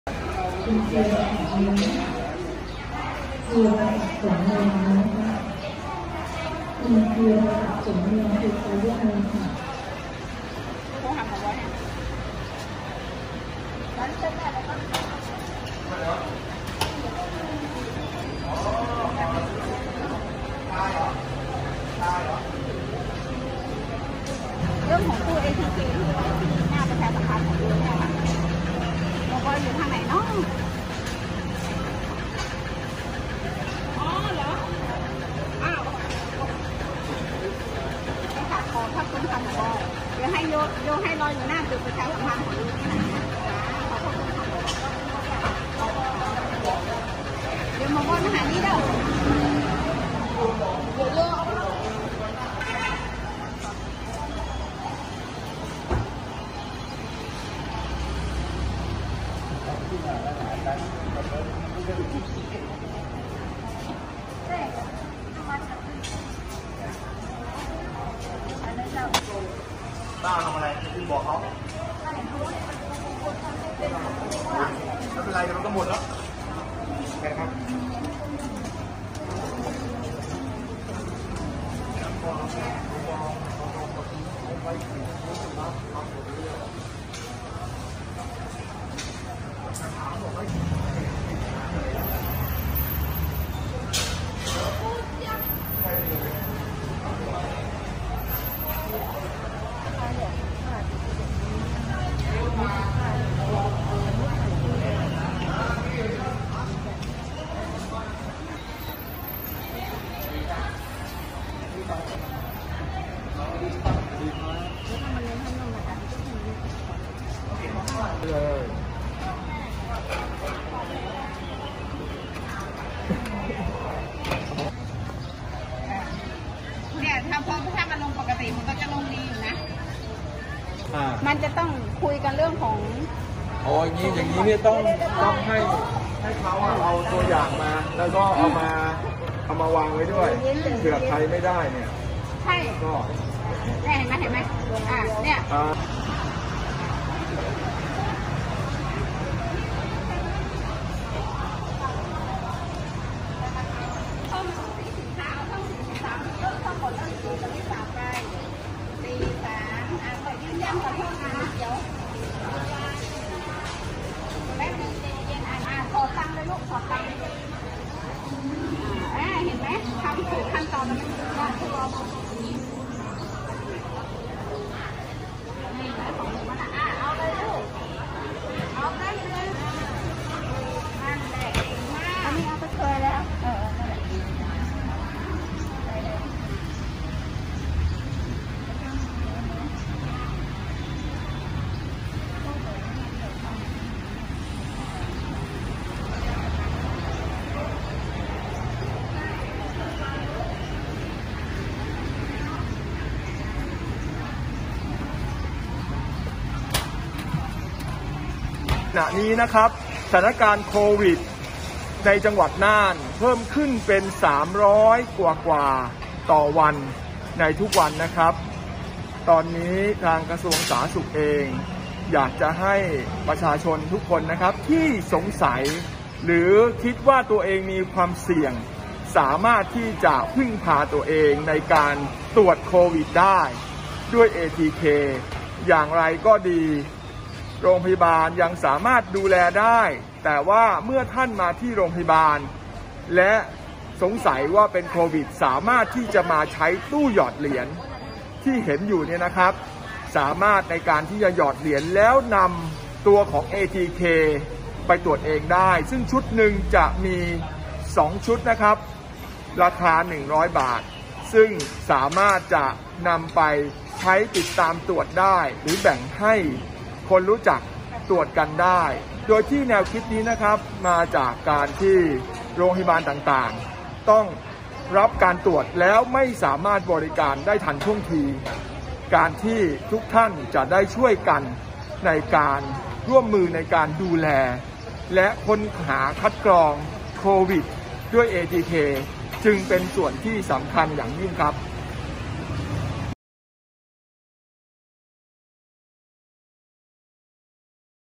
รพ.น่าน ไอเดียเก๋ เปิดตู้จำหน่าย ATK อัตโนมัติ
โรงพยาบาลน่าน ตำบลในเวียง อำเภอเมืองน่าน จังหวัดน่าน เปิดตัว ตู้จำหน่าย ATK อัตโนมัติ ลดความเสี่ยง ลดเจ้าหน้าที่ โดยเครื่องดังกล่าวเป็นการดัดแปลงมาจากเครื่องขายสินค้าอัตโนมัติ โดยเป็นที่ได้รับความสนใจจากประชาชนเป็นจำนวนมาก